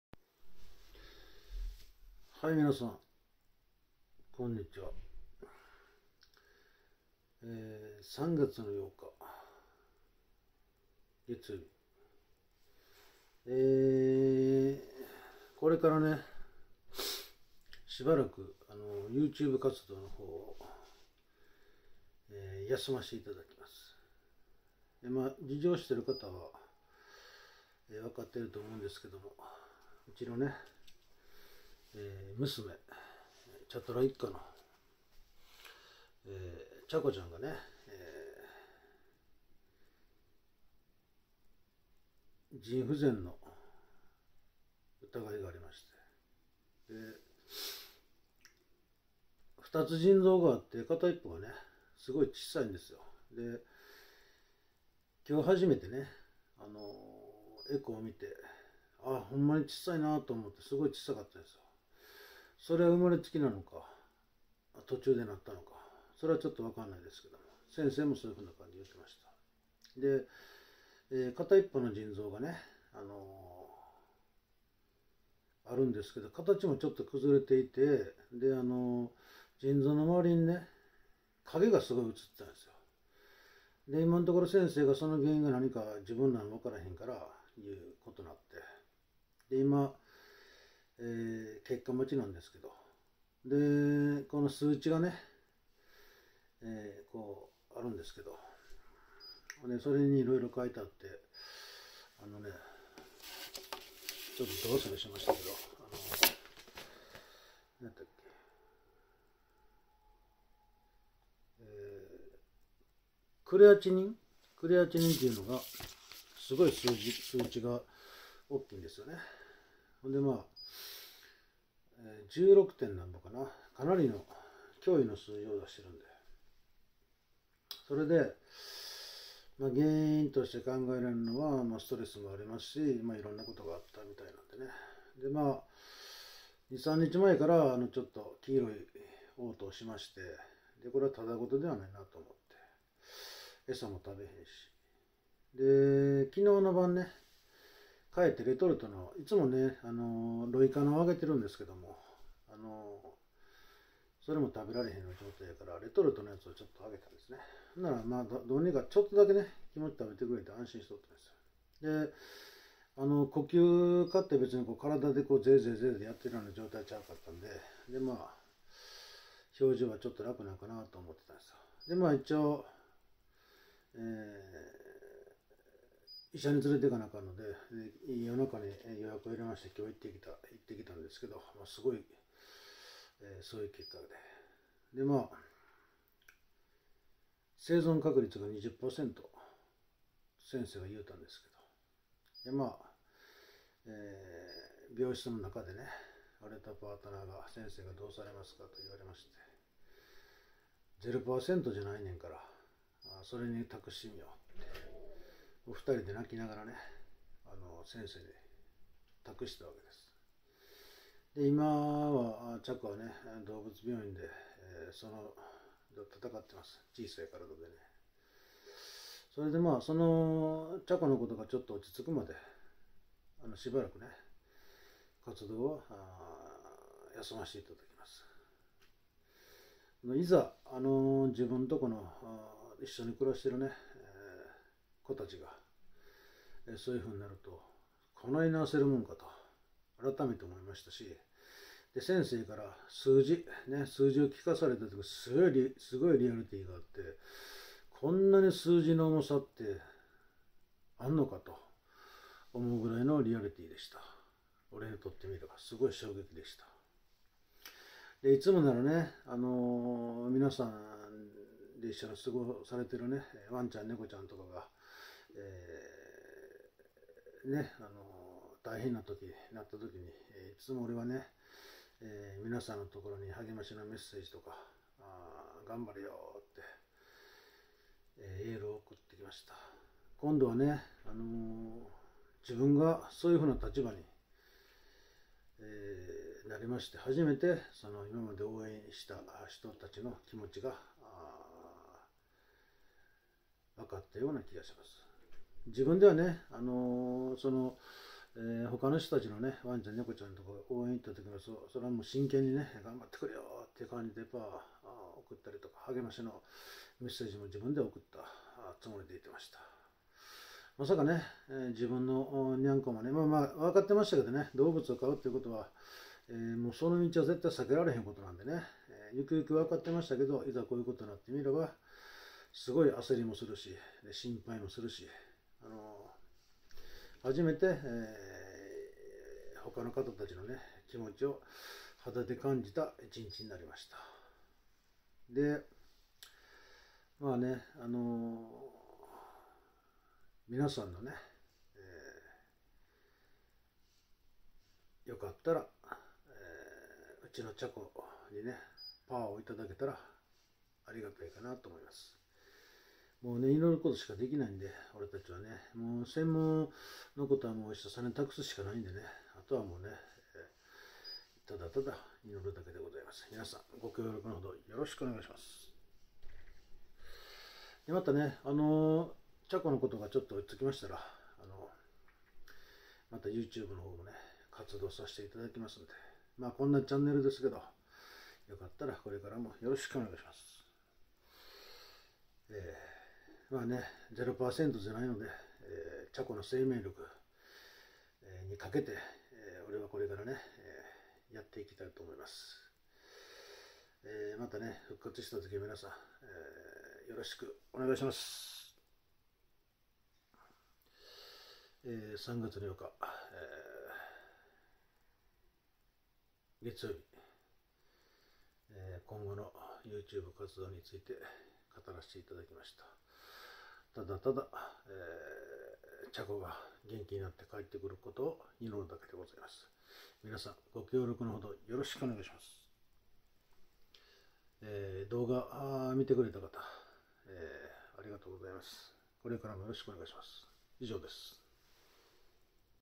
皆様へ YouTube活動をしばらく休止させて頂きます！
娘 茶娘ちゃんが腎不全の疑いの為入院しました！
数値が非常に高く命に関わる問題です！娘 茶娘の為に今してあげられる事は無事に元気な姿で帰って来る事を願うばかりです！皆様のパワーを頂けまたらありがたいです！

コメントに対する返信は少し遅れるかも知れないですが必ず返信しますのでよろしくお願い致します！